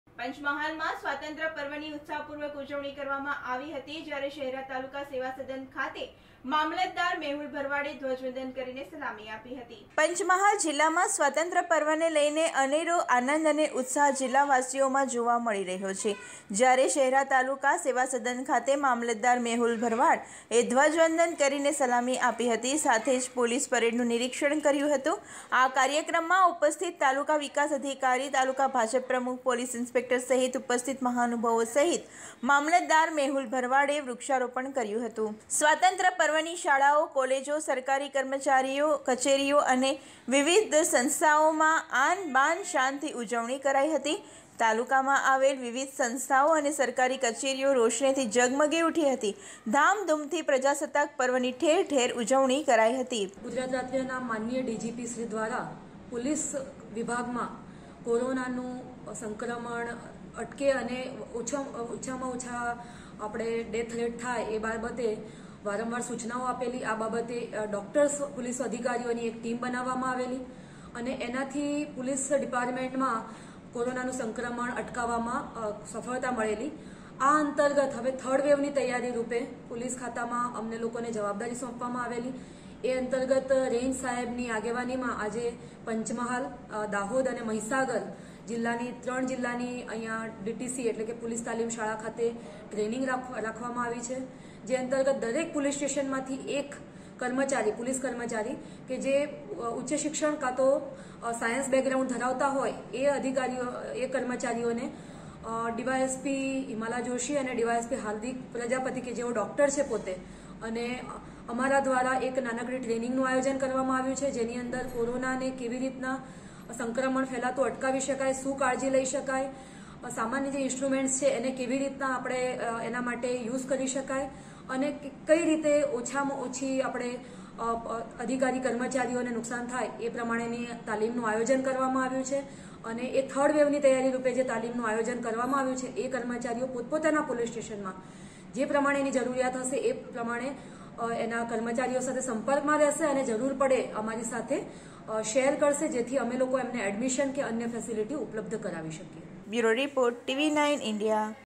પંચમહાલમાં સ્વાતંત્ર પર્વની ઉત્સાહપૂર્વક ઉજવણી કરવામાં આવી હતી...|TV9INDIA GUJARATI
પંચમહાલ જિલ્લામાં સ્વાતંત્ર પર્વને લઇને અનેરો આનંદ અને ઉત્સાહ જિલ્લાવાસીઓમાં જોવા મળી રહ્યો હતો. જ્યારે શહેરા  તાલુકા સેવાસદન ખાતે મામલતદાર મેહુલ ભરવાડ એ ધ્વજ વંદન કરીને સલામી આપી હતી.સાથે પોલીસ પરેડનું નિરીક્ષણ કરેલ હતુ. આ કાર્યક્રમમાં ઉપસ્થિત તાલુકા વિકાસ અધિકારી, તાલુકા ભાજપ પ્રમુખ, પોલીસ ઇન્સ્પેક્ટર સહિત ઉપસ્થિત મહાનુભાવો સહિત મામલતદાર મેહુલ ભરવાડ એ વૃક્ષારોપણ કરેલ હતું.સ્વાતંત્ર પર્વની શાળાઓ, કોલેજો, સરકારી કચેરીઓ અને વિવિધ સંસ્થાઓ મા આન બાન શાન થી  ઉજવણી કરવામાં આવી હતી.તાલુકામા આવેલ વિવિધ સંસ્થાઓ અને  સરકારી કચેરીઓ રોશનીથી ઝગમગી ઉઠી હતી.  ધામધૂમથી પ્રજાસતાક પર્વ ની ઠેર ઠેર  ઊજવણી કરાઇ હતી.